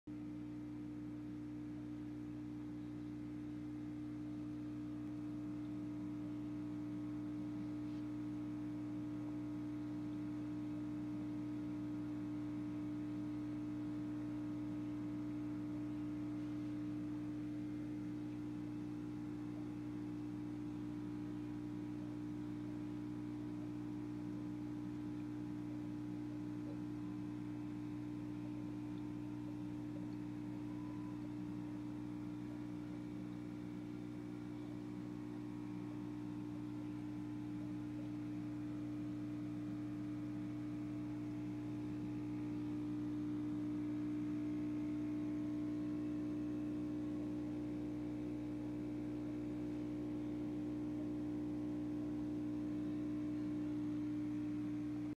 Red Oscar, Fire Eel, SRD Flowerhorn, Silver Arowana & Sevrums
125 Gallon South American Cichlid tank stocked with

Ruby the Red Oscar
Fire Eel
Silver Arowana
SRD Flowerhorn
Turquoise Sevrums
Gold Sevrums (Red Spotted)
Silver Dollars